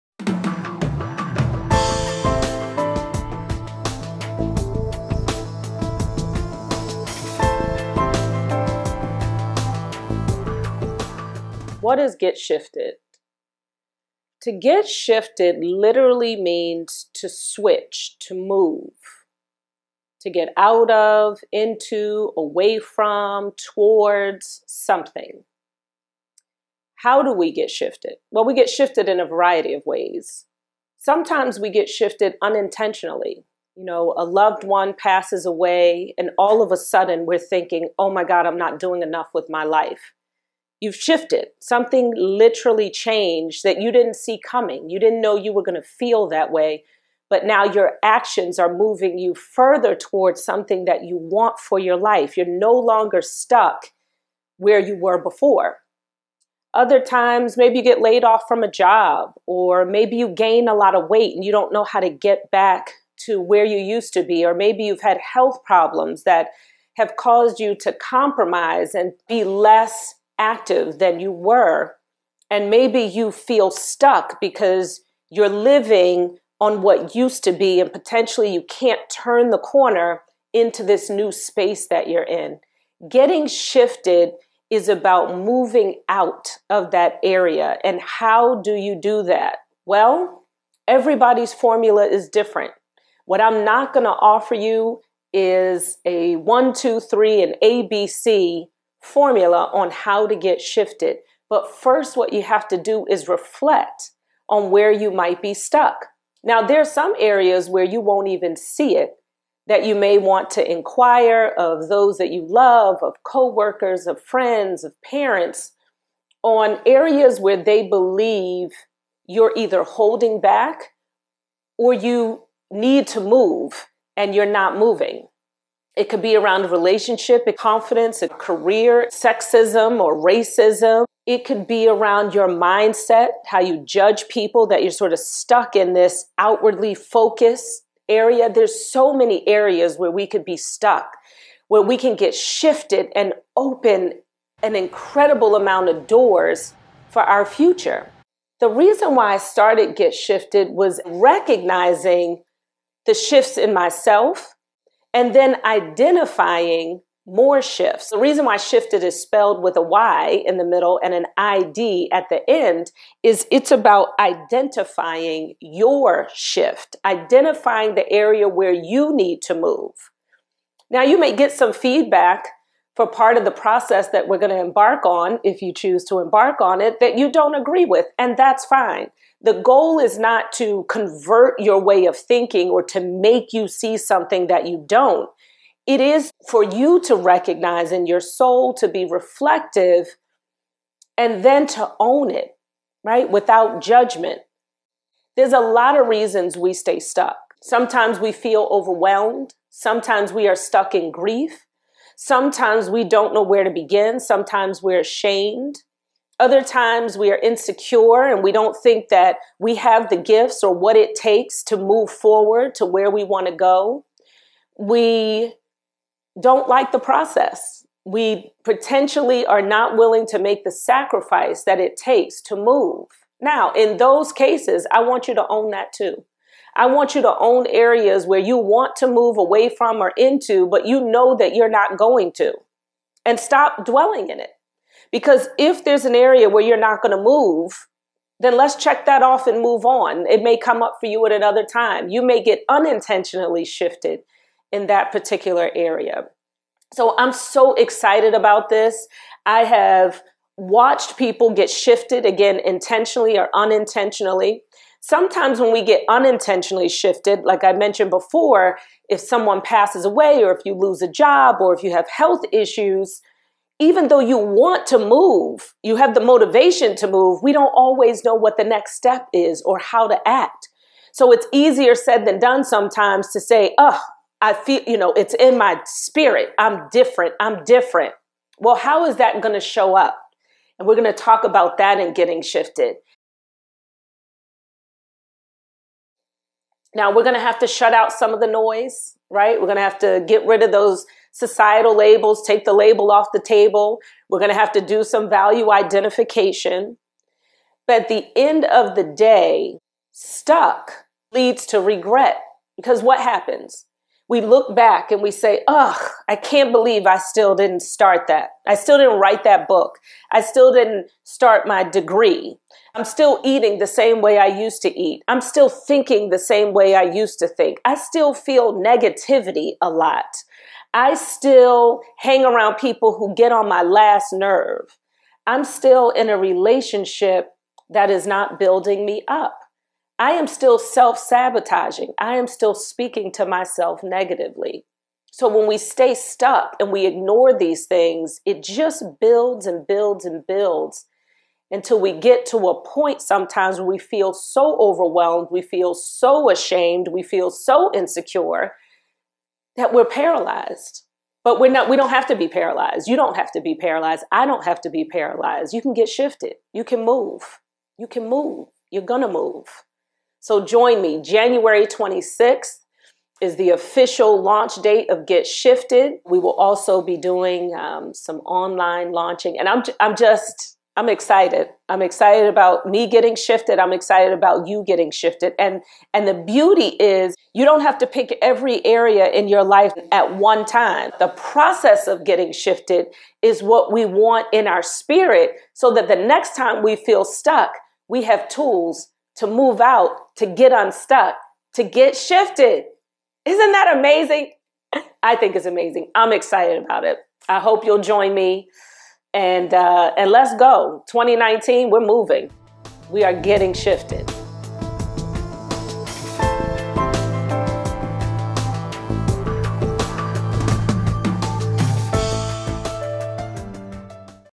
WHAT IS SHYFTID™?
Monica Cost, President of The Core Value Company talks about her new SHYFTID™ movement poised to get thousands of people unstuck and on their way to a more fulfilling existence with less regret.